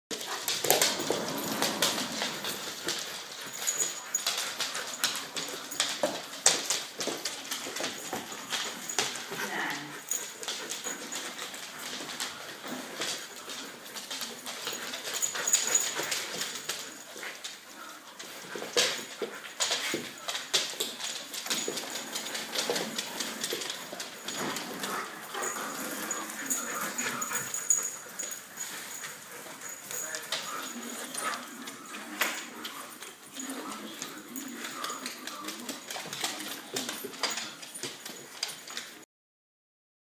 Hunde Silvester 2014 - Hundepension
Und so geht es Silvester bei uns zu, denn bei uns wird nicht geböllert und alle Halter sind froh, an Neujahr einen völlig entspannten Hund zu begrüssen.
Mit Originalton :-) Ist wirklich so. Kein Gekläffe und/oder Gezicke.